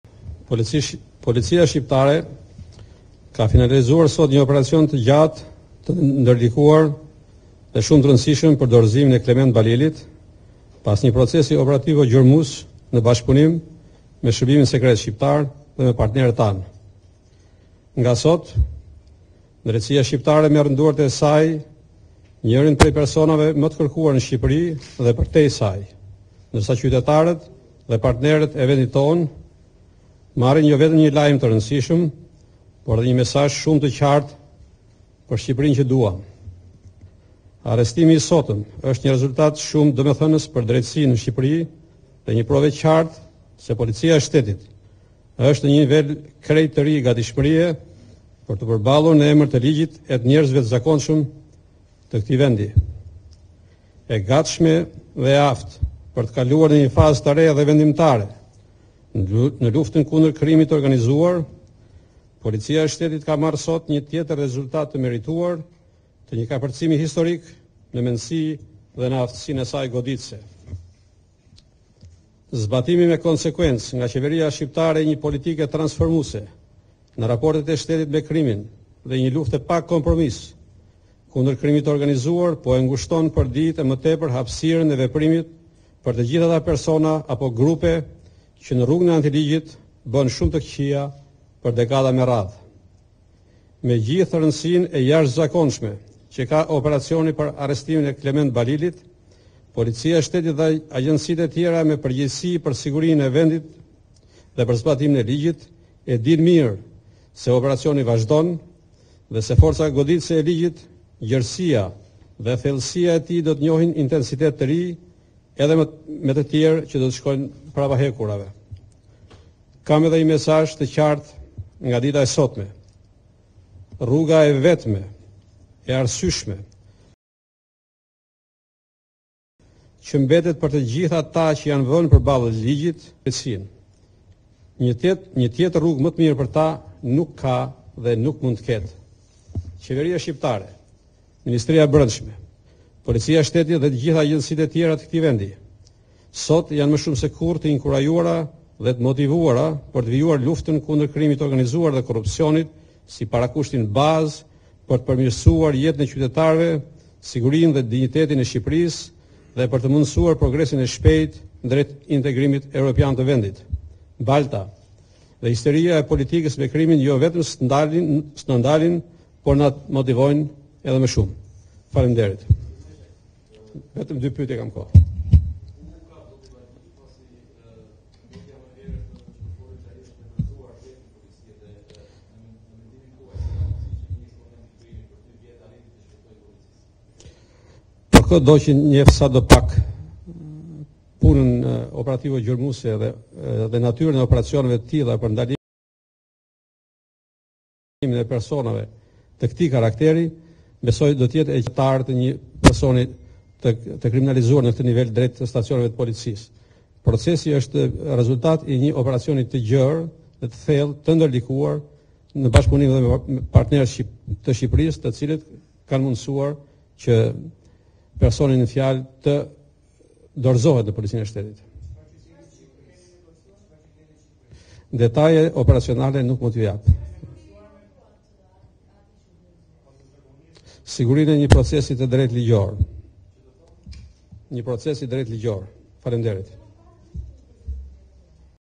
Report TV - Pas dy vitesh në arrati, Balili në pranga, Lleshaj: Kemi në duar njeriun më të kërkuar
Zyrtarisht i shumëkërkuari Klement Balili është në prangat e Policisë së Shtetit. Ministri i Brendshëm Sandër Lleshaj përmes një deklarate për mediat njoftoi se ‘Escobari i Ballkanit’ i akuzuar për trafik ndërkombëtar të lëndëve narkotike është vetëdorëzuar. ‘Peshku i madh’ apo ‘Magjistari’ sikur është cilësuar nga ish-ambasadori i SHBA në Tiranë, Donald Lu vendosi që pas dy vitesh në arrati të përballet me drejtësinë.

‘Nga sot policia shqiptare merr në duart e saj një prej personave më të kërkuar, partnerët tanë marrin një lajm të rëndësishëm dhe mesazh të qartë. Nga sot drejtësia shqiptare merr në duar të shumëkërkuarin Klement Balili. Policia e Shtetit është në nivel të lartë gatishmërisë në luftën kundër krimit të organizuar. Një tjetër rrugë më të mirë për ta nuk ka dhe nuk do të ketë, të gjithë jemi të motivuar për të vijuar luftën kundër krimit të organizuar dhe korrupsionit.” tha ai.

Fillimisht lajmin për dorëzimin e Balilit e dha ish-kryeministri Sali Berisha paraditen e sotme, ndërsa rreth dy orë më pas, Lleshaj e konfirmoi. “Dorezohet Klement Balili! Qytetari dixhital informon. Pershendetje doktorin vetdorezohet kelment Balili ne prokurorin e krimeve te renda ai esht nisur drejt tiranes me Rigels Balilin” shkruate Berisha. Por kush është Klement Balili?